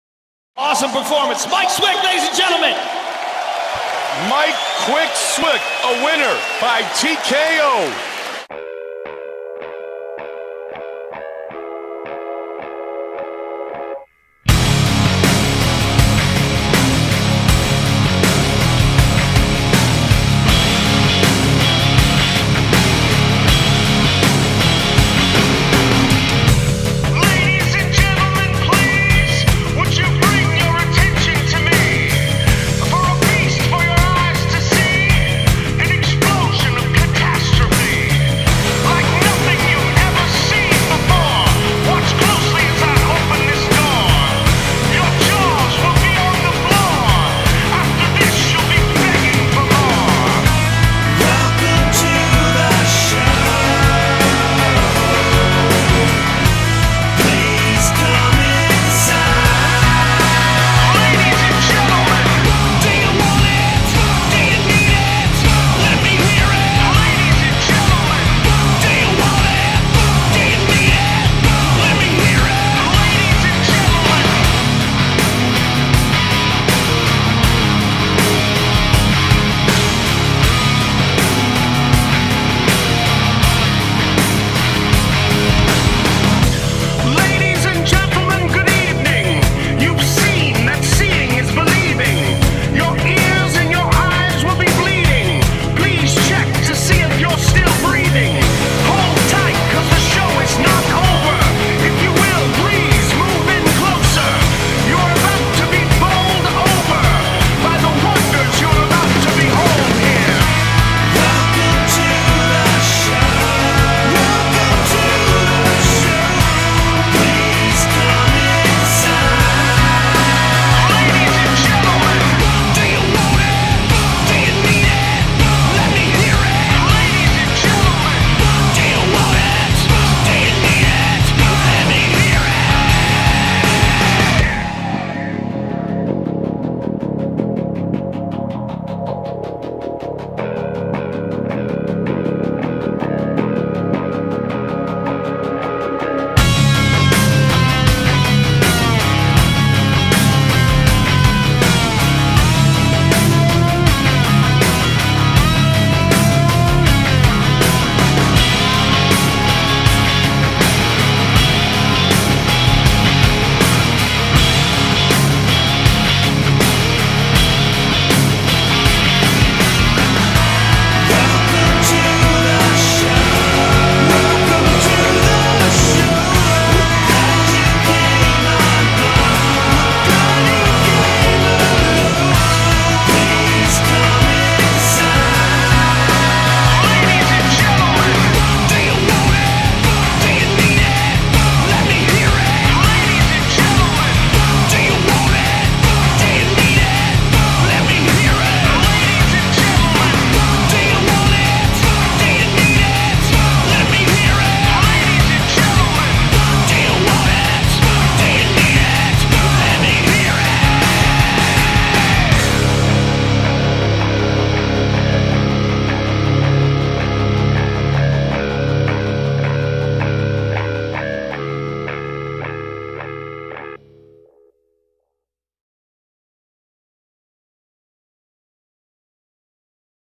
Mike Swick Fight Highlight - Mikes Official YouTube Is OfficialSwick - TSM Studios
This is Mike's fight highlight updated through 2010. Edited by TSM.